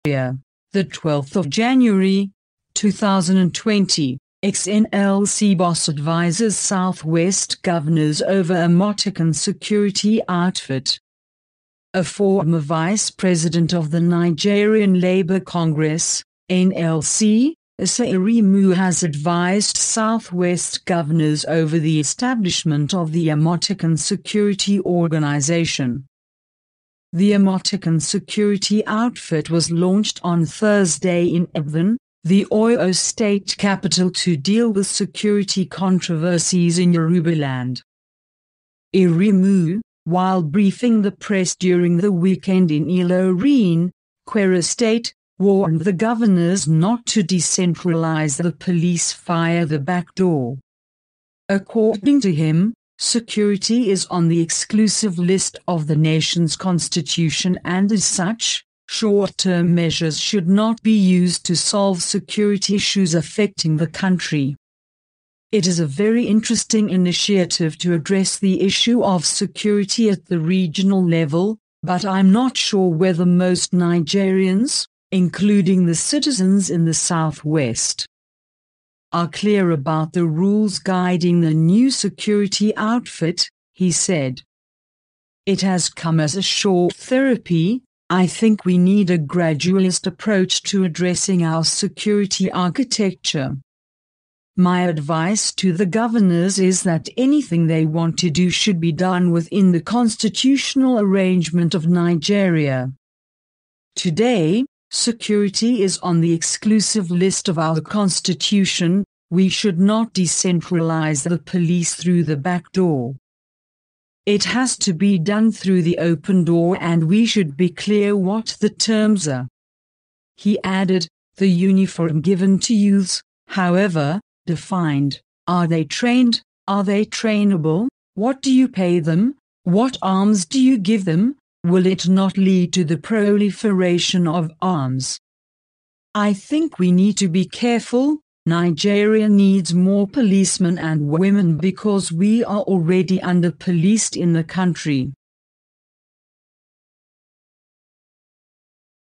Ex-NLC Boss Advises South-West Governors Over Amotekun Security Outfit
A former Vice President of the Nigerian Labour Congress (NLC) Issa Aremu has advised South-West governors over the establishment of the Amotekun security Organization.
The Amotekun security outfit was launched on Thursday in Ibadan, the Oyo State capital to deal with security controversies in Yorubaland.
Aremu, while briefing  the press during the weekend in Ilorin, Kwara State, warned the governors not to decentralize the police via the backdoor.